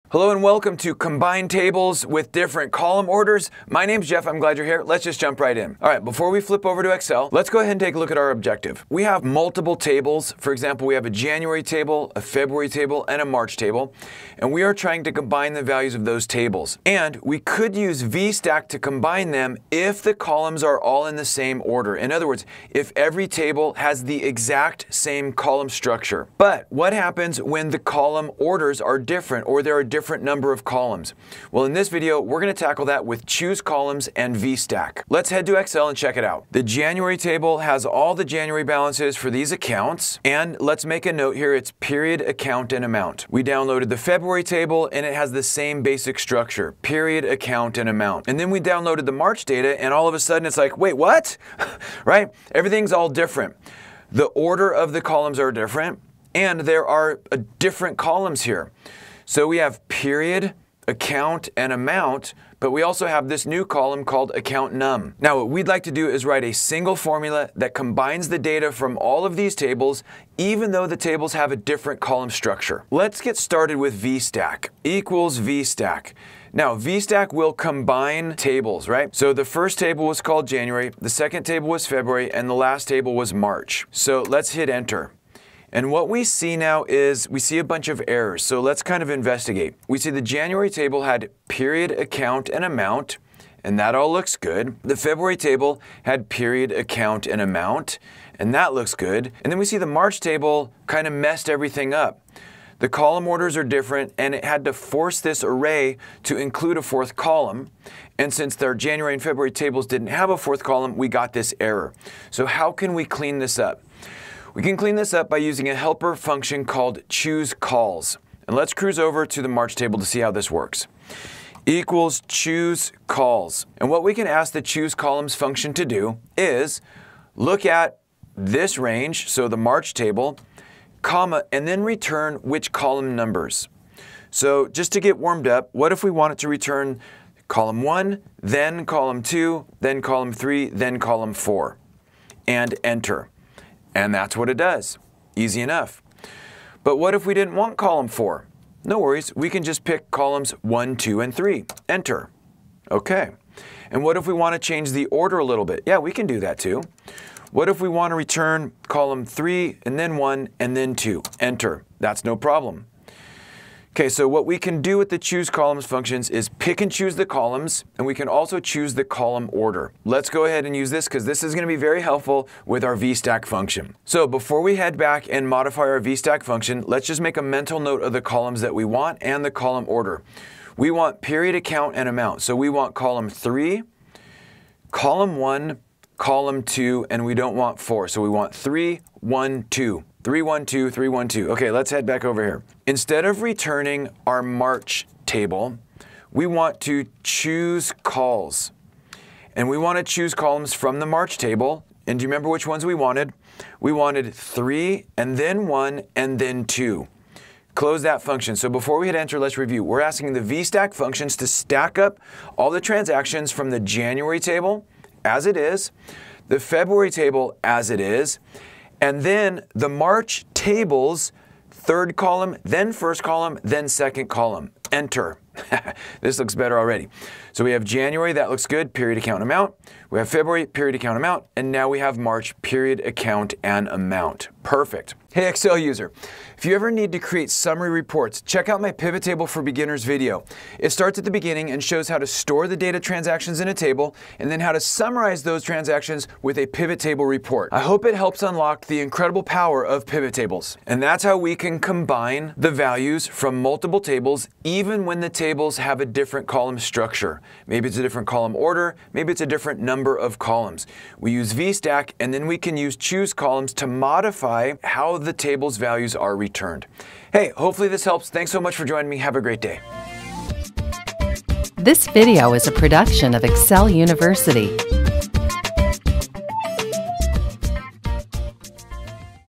Excel How To Combine Tables with Different Column Orders
In this video, we learn how to combine tables even when they have a different column structure. They could have a different column order, or even a different number of columns. We use VSTACK along with CHOOSECOLS to do so.

Chapters:
00:00 - Introduction
00:35 - VSTACK
01:59 - CHOOSECOLS